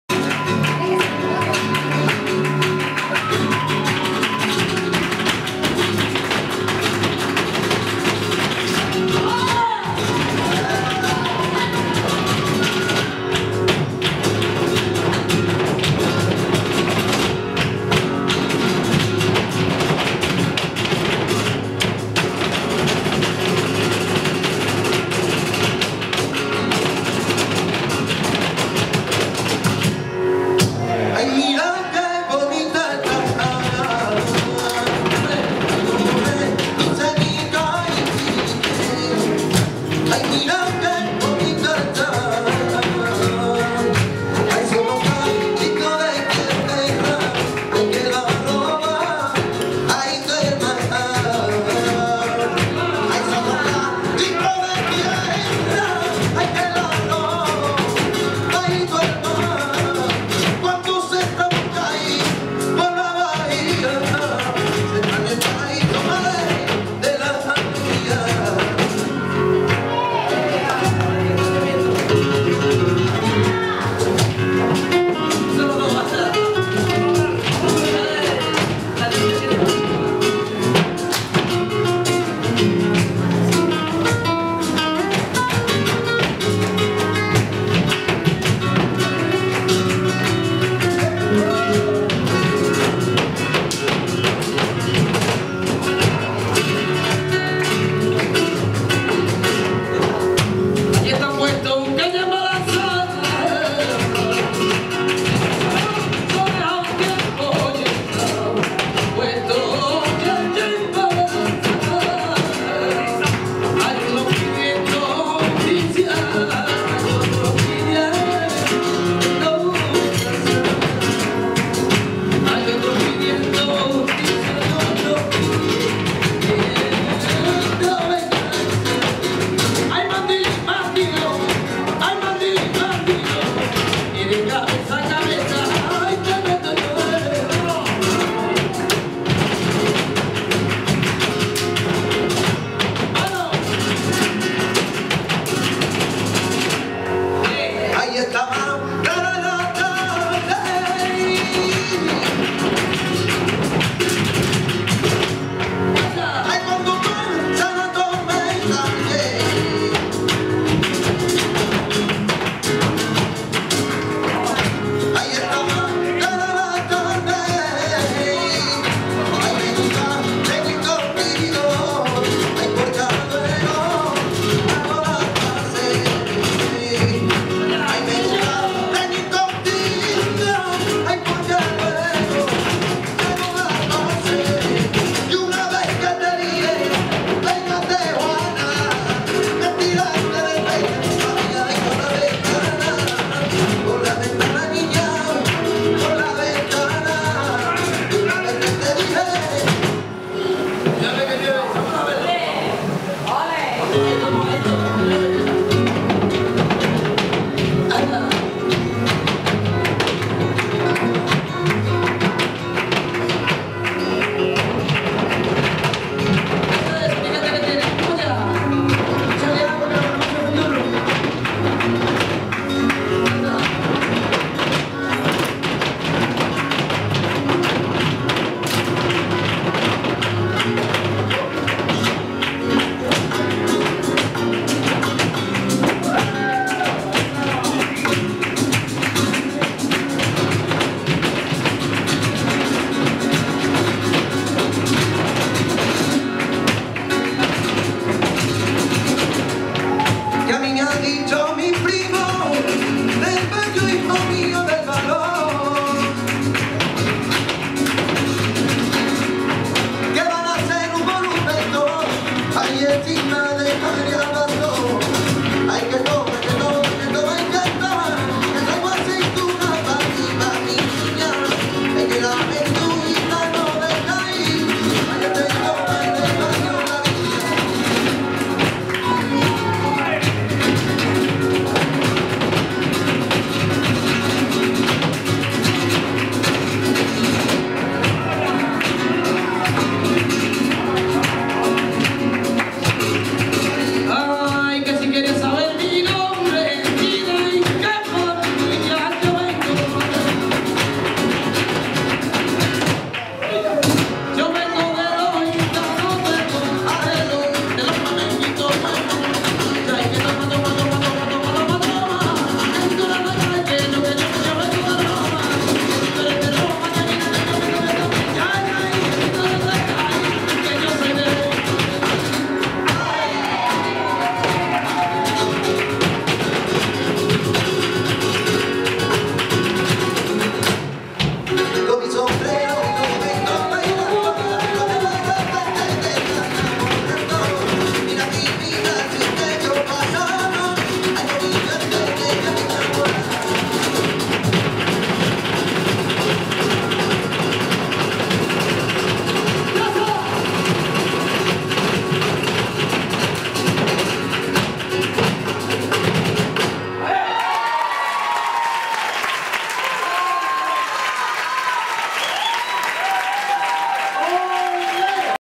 Alegrías con Silvana Perdomo, Eva Iglesias, Diego Ferreira y Adrian Verges - Tablao Perro Andaluz
Baile: Silvana Perdomo, Eva Iglesias, Diego Ferreira y Adrian Verges
Cante: Eugenio Romero, Maximiliano Serral
Guitarra: Allier Diaz Ferrer, Sebastian Espeche
Caja: Papu Gigena
Flauta: Augusto Reinhold

10 de diciembre 2011, Buenos Aires